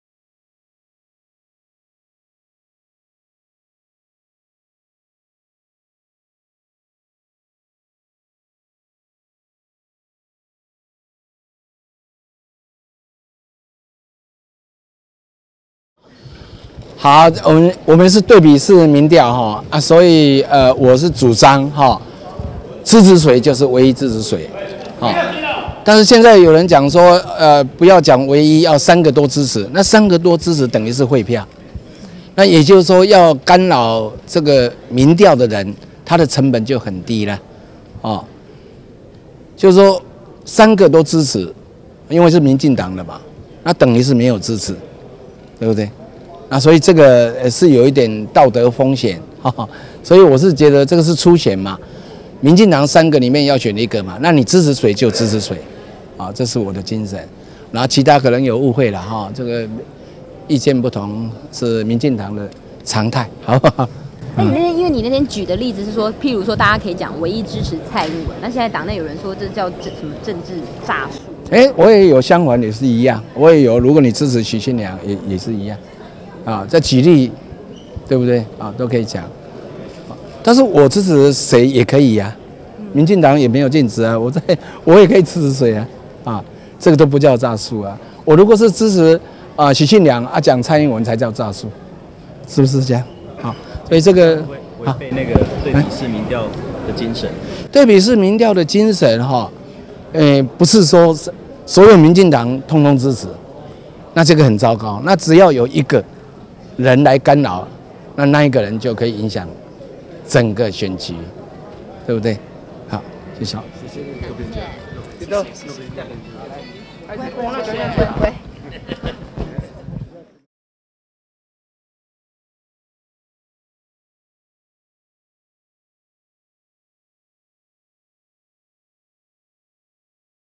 民進黨 2012總統大選初選第一場政見發表會-謝長廷受訪
民進黨今天（4/9）日舉行2012年總統大選初選第一場政見發表會，三位候選人蘇貞昌、許信良、蔡英文依抽籤順序抵達會場，民進黨總統初選將辦四場電視政見發表會，首場今日下午 2到 4時登場。政見會分申論、提問與回答、結論三階段，由前陸委會主委吳釗燮、台灣智庫董事長陳博志、政大教授顧忠華提問，申論與結論各 10分鐘。

政見會場設在台北國際會議中心，會場舞台設計是以民進黨傳統的大地綠為主色，舞台背景是一幅飛揚的民進黨黨旗，中間則是鑲著世界地圖。根據抽籤順序，許信良站舞台正中間，蘇貞昌、蔡英文在兩側，政見會開始前由民進黨代理主席柯建銘致詞

三位候選人也各有支持者到場為他們加油，數位首投族的青年學生，手心上貼著蔡英文貼紙高喊『台灣需要新世代 世代交替真超越』為蔡英文加油。蘇貞昌則是由超越志工手持『自信 台灣』等標語為他加油，許信良則是由夫人陪同出席。三位候選人各自帶二十五位貴賓進場聆聽，政見發表會準時於下午二點登場。

政見發表會前，前行政院長謝長廷受訪被問到近來民調說法一事，謝長廷表示這是對比式民調，因此他主張支持誰就是唯一支持誰，現在有人說不能講唯一要講三個都支持，謝長廷說如果說三個都支持就等於是廢票，也就是說要干擾民調的人他成本就很低。

謝長廷認為，三個都支持因為都是民進黨的候選人，所以等於是沒支持，這是有道德風險的，他認為這是初選，民進黨是要三個人選選一個，支持誰就是支持誰，這是他的精神，其他說法可能有人誤會，但是民進黨有許多意見不同這是常態。

對於提到『唯一支持』蔡英文，有黨內人質疑這是詐術，謝長廷表示，他也有其他舉例比如支持許信良就是唯一支持許信良，這舉例都可以講，而他要支持誰也是可以講，民進黨並沒有規定他不能講他要支持誰，如果他支持許信良卻故意講蔡英文，這才叫做詐術。

謝長廷認為，對比式民調的精神，不是說所有民進黨都支持，那只要有一個人來干擾，那麼這個人就可以影響整個選情。